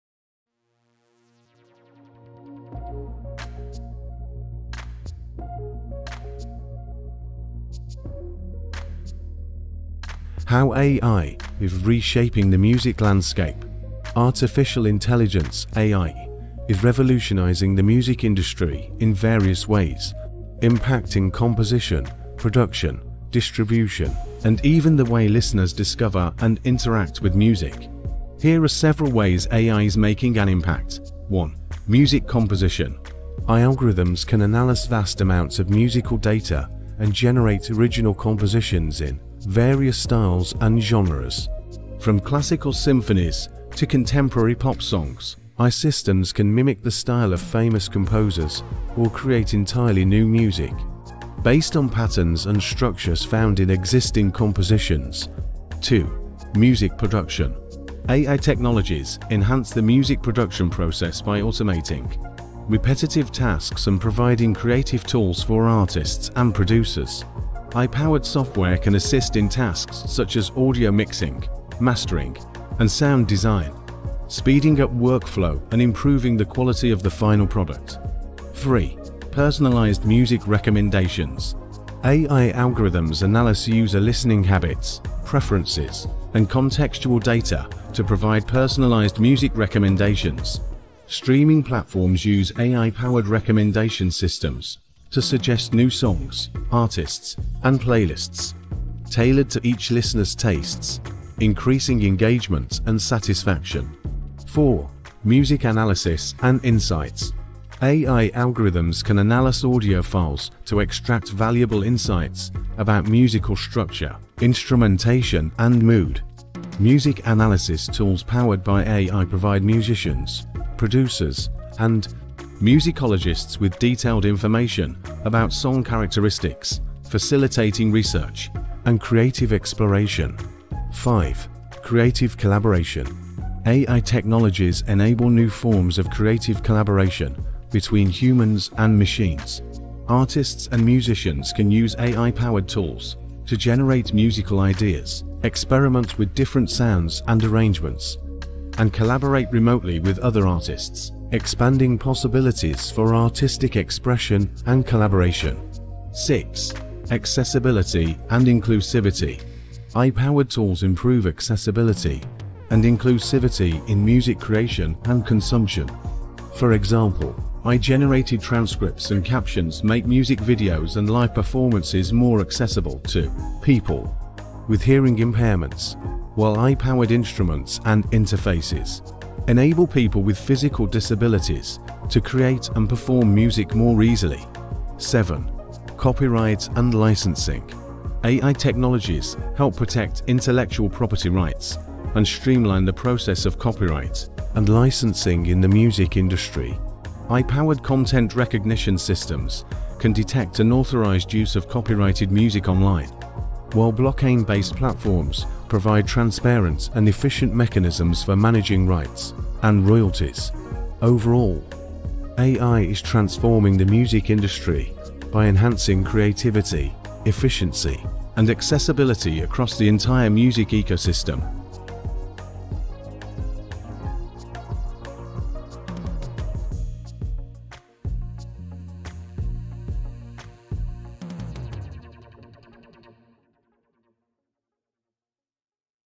Harmonizing Innovation: How AI is Reshaping the Music Landscape
In this video, dive into the melodies of tomorrow and embrace the role of artificial intelligence in driving musical evolution. Delve into AI-powered composition, production, and collaboration, witnessing how technology is fuelling creativity and pushing the boundaries of musical experimentation. Join the conversation on the intersection of art and technology, where innovation harmonizes with tradition to shape the sound of the future.

Explore the transformative power of artificial intelligence in revolutionizing music creation, production, and consumption. From AI-generated compositions to personalized recommendations, discover how innovative technologies are harmonizing creativity and technology to shape the future of music. Join us on a journey through the evolving music landscape, where AI is unlocking new possibilities and redefining the boundaries of artistic expression.
Step beyond notes and beats to explore how artificial intelligence is enhancing music accessibility and inclusivity. From AI-generated captions to adaptive instruments, witness how technology is breaking down barriers and empowering individuals of all abilities to create, perform, and enjoy music. Join us in celebrating the transformative power of AI in fostering a more inclusive and diverse musical landscape for all.